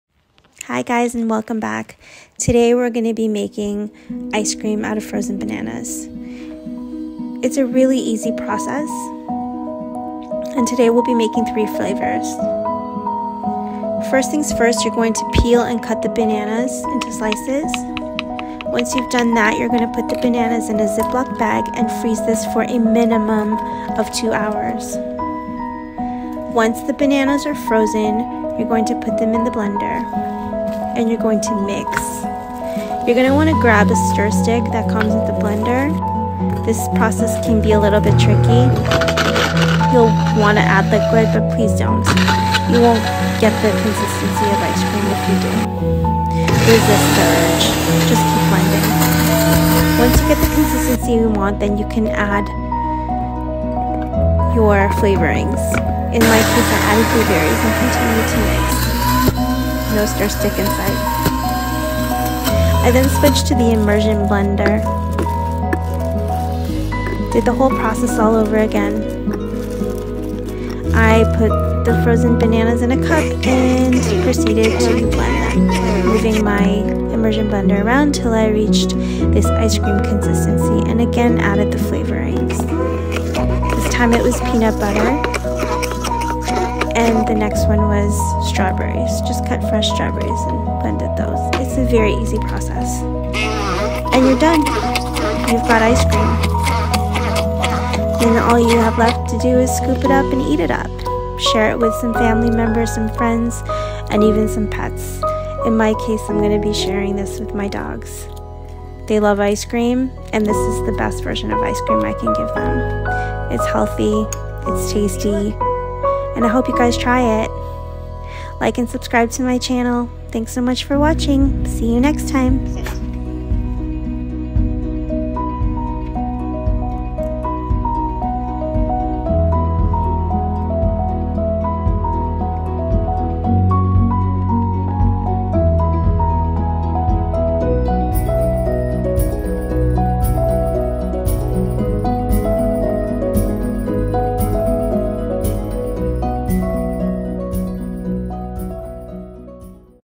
Ice Cream Made From Fruits? The Results Will Amaze You | Easy and Only 2 Ingredients 🍌
In this video we’ll be making a simple ice cream made from frozen bananas. There are many variations of flavors you can make with this, the possibilities are endless, but today I made my favorites. This is dairy free, egg free with no added sugars or preservatives. It is a wholesome treat that is nutritious for humans and for dog consumption  as well (as long as the peanut butter you use is xylitol free). The consistency is like ice cream when eaten right away.  Give it a try if you haven't yet, your mind will be blown :)